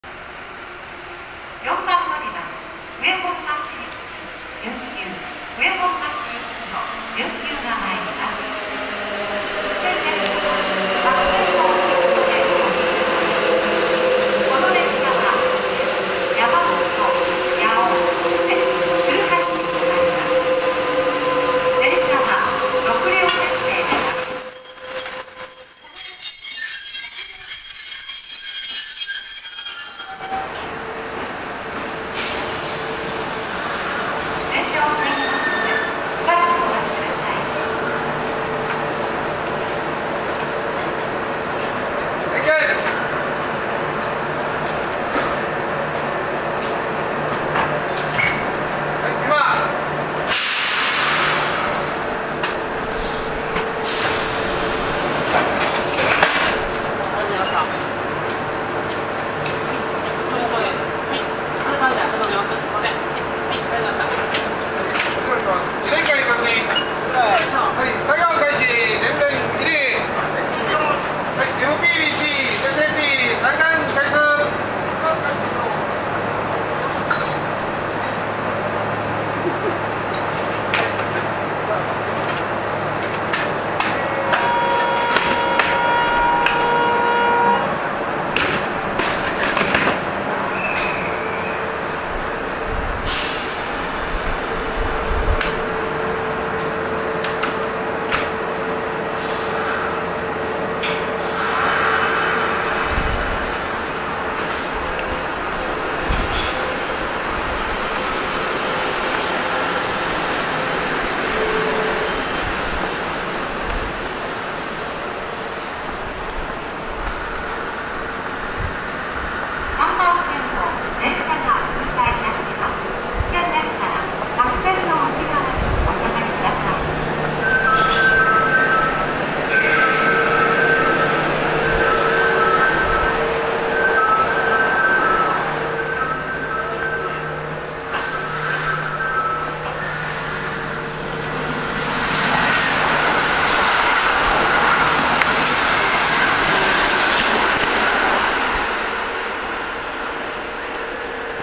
高安駅 連結作業 ＊2010年1月28日録音
高安駅の連結作業です。